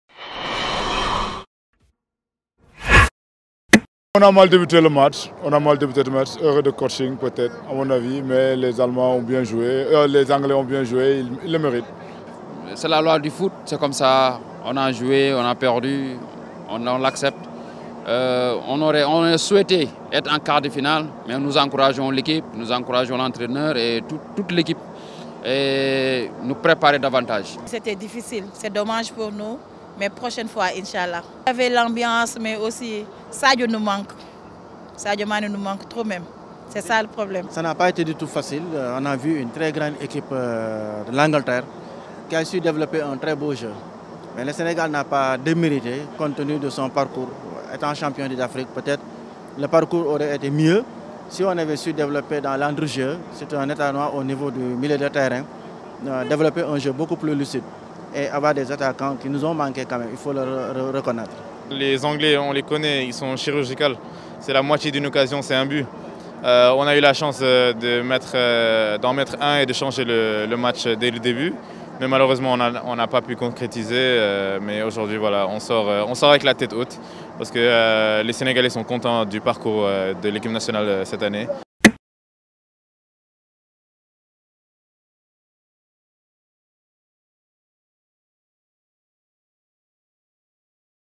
حسرة الجماهير السنغالية بعد الإقصاء أمام إنجلترا
أعرب جمهور المنتخب السنغالي لكرة القدم، في حديثه مع موقع Le360سبور عن حسرته بعد الإقصاء أمام منتخب إنجلترا وعدم التأهل إلى دور ربع نهائي كأس العالم، وذلك خلال المباراة القوية التي جمعت بينهما على أرضية ملعب البيت، لحساب دور ثمن نهائي "المونديال".